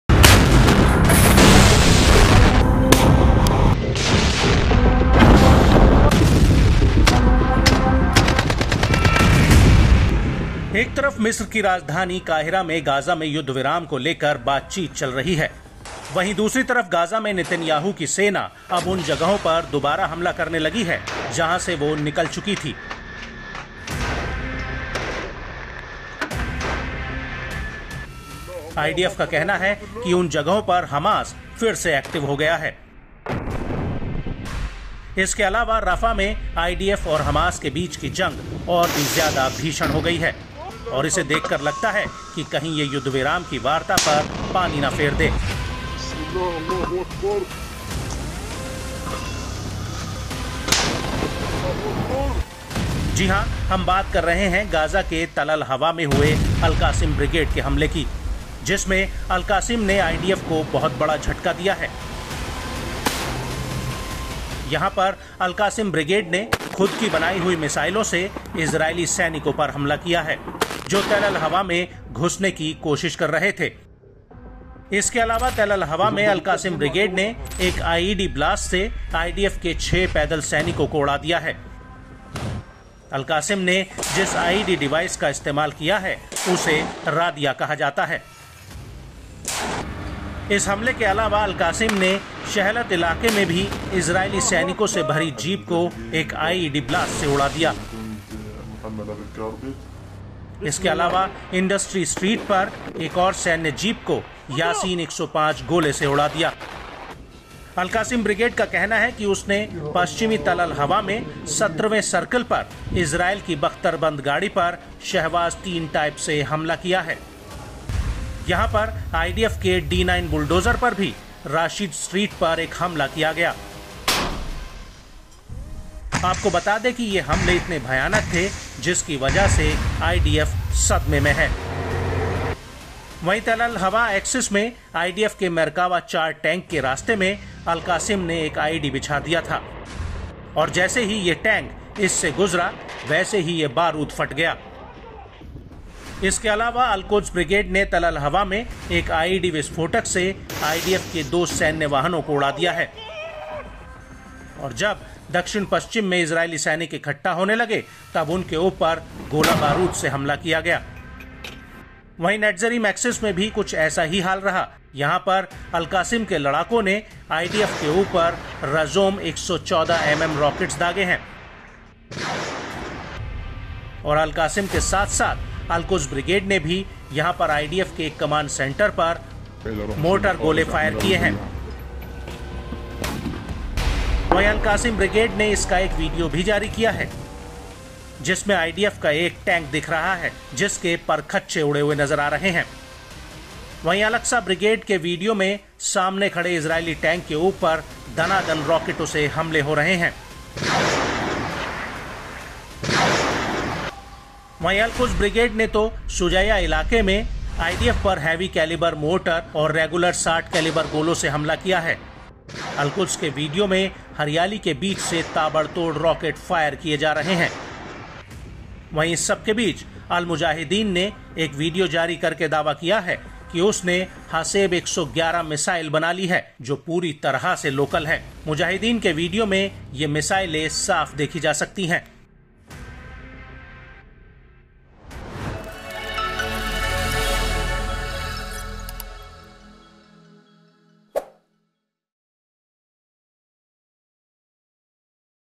Israel Hamas War : हमास ने बनाई खुद की मिसाइल, इज़रायल में मच गई तबाही | Palestine| Netanyahu | N18G
In the midst of the Israel-Hamas War, the Al-Qassam Brigades blew up 6 IDF infantrymen with an IED blast in Tel al-Hawa, Gaza. The IED device used by the Al-Qassam Brigades is called Ra'diya. Apart from this attack, the Al-Qassam Brigades also blew up a jeep full of Israeli soldiers with an IED blast in the Shelehat area.
Israel Hamas War के बीच Gaza के Tel al-Hawa में Al-Qassam Brigades ने एक IED ब्लास्ट से IDF के 6 पैदल सैनिकों को उड़ा दिया है. Al-Qassam Brigades ने जिस IED डिवाइस का इस्तेमाल किया है उसे Ra'diya कहा जाता है. इस हमले के अलावा, Al-Qassam Brigades ने Shelehat इलाके में भी Israeli Soldiers से भरी जीप को एक IED ब्लास्ट से उड़ा दिया है. इसके अलावा, Industry Street पर एक और सैन्य जीप को Yassin 105 गोलों से उड़ा दिया गया है.